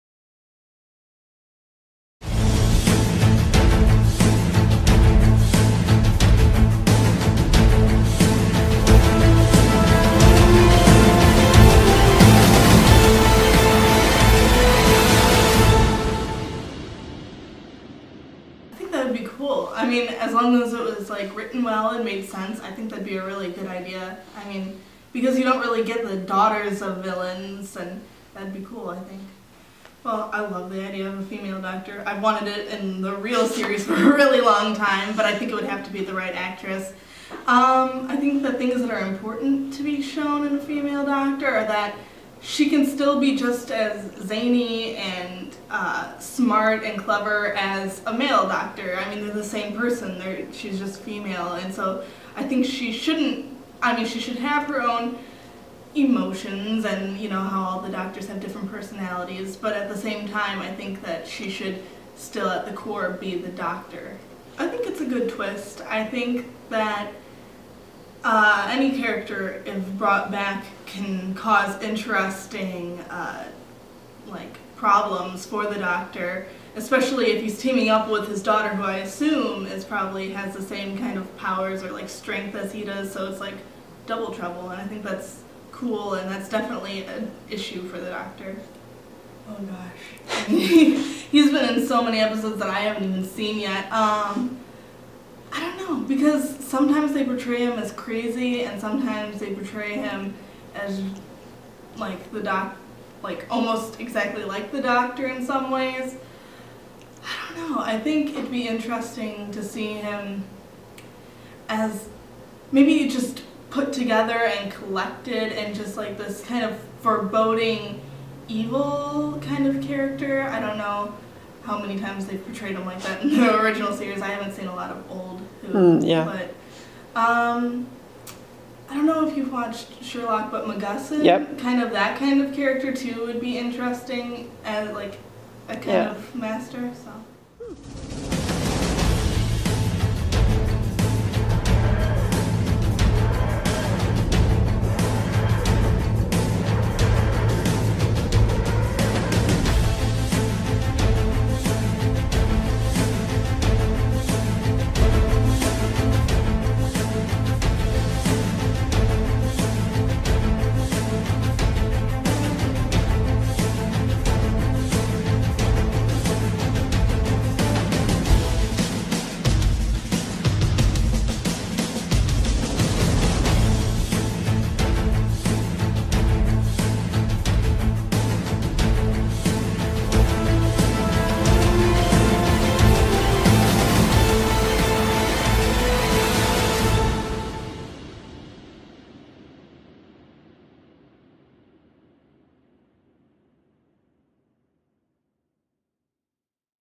A New Era Series 2: Leighton Williams BONUS Interview
This is a bonus interview with Leighton Williams, who plays Hannah in Series 2 of A New Era.